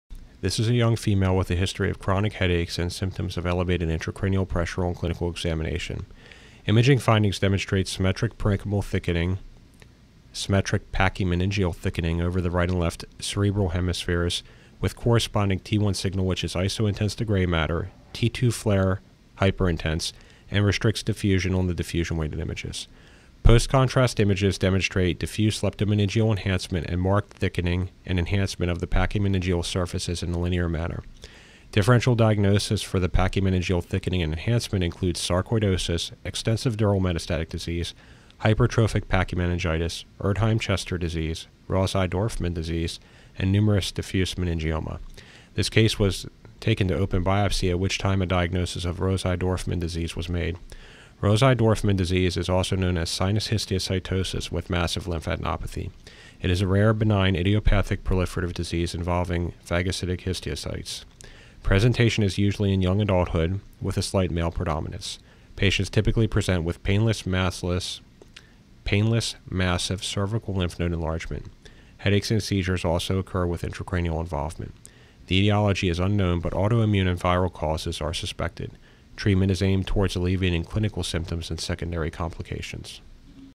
Rosai Dorfman
33-year-old female with history of chronic headaches and symptoms of elevated intracranial pressure on clinical examination. Images demonstrate symmetric pachymeningeal thickening over the right and left cerebral hemispheres with corresponding T1 signal which is isointense to gray matter, T2/FLAIR hyperintense, and restricts diffusion on the DWI images. Postcontrast images demonstrate diffuse leptomeningeal enhancement with marked thickening and enhancement of the pachymeningeal surfaces. Differential diagnosis sarcoidosis, extensive dural metastatic disease, hypertrophic pachymeningitis, Erdheim-Chester disease, Rosai-Dorfman disease and numerous diffuse meningioma. This case went to open biopsy at which time a diagnosis of Rosai-Dorfman disease was made. Rosai-Dorfman is a rare benign idiopathic proliferative disease. Presentation is usually in young adulthood with a slight male predominance. Patient's typically present with painless massive cervical lymph node enlargement. Headaches and seizures also occur with intracranial involvement. The etiology is unknown but autoimmune and viral causes have been suspected. NMR 328